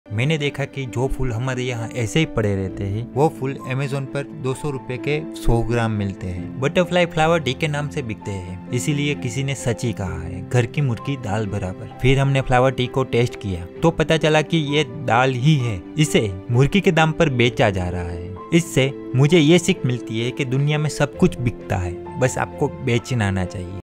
Butterfly Pea Flower Tea & Blue Pea Tea testing
Mene dekha ki jo hmare yaha aese pade rahte ful .

Wo ful Amazon par 200  ke 100 gm milte he.

Butterfly  Flower Tea  ke name se bik te hai. Eis lye to kisi ne sach hi kaha hai. Ghare ki murkhi dal barabar. Fir Hume ne flower tea ko test Kiya to pata chala ki ye dal hi he . Ise murkhi ke dam par becha jata hai.

Ise muje ye Sikh mili ke is duniya me sab kuch bik ta hai bs apko bechna aana chahiye.

Camera canon m50

Go pro 9

Mic rode.

Voice over  mic